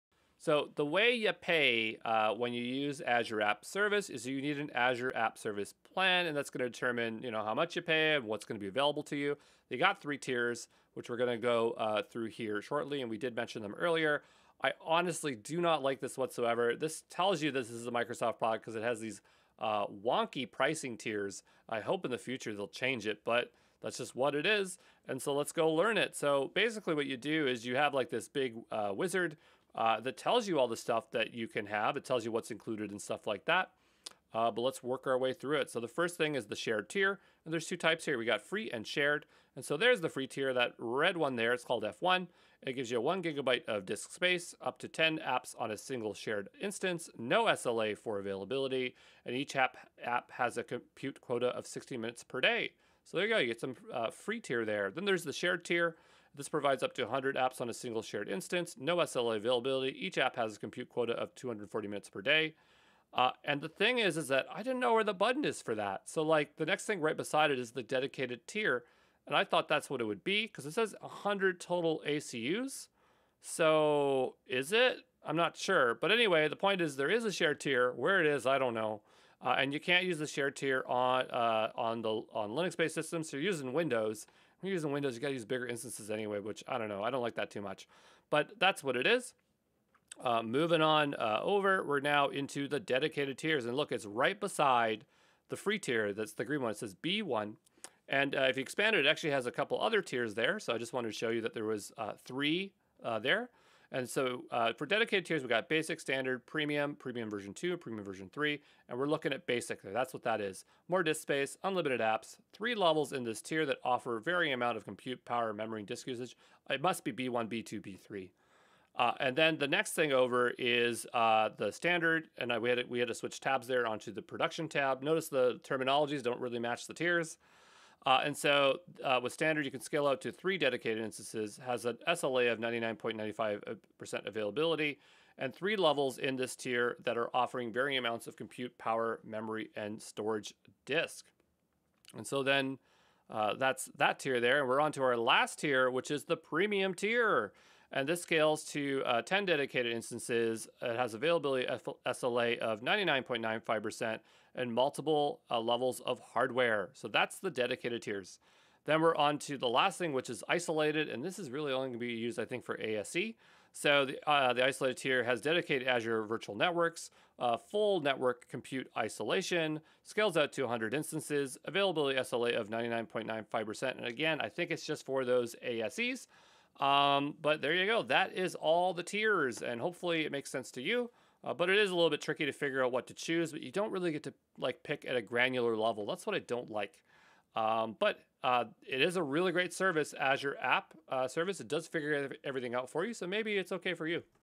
Choosing the Right Azure App Service Plan - Azure Associate Course
Explore the different Azure App Service plans and how to choose the right one based on your app’s performance, scaling, and cost requirements.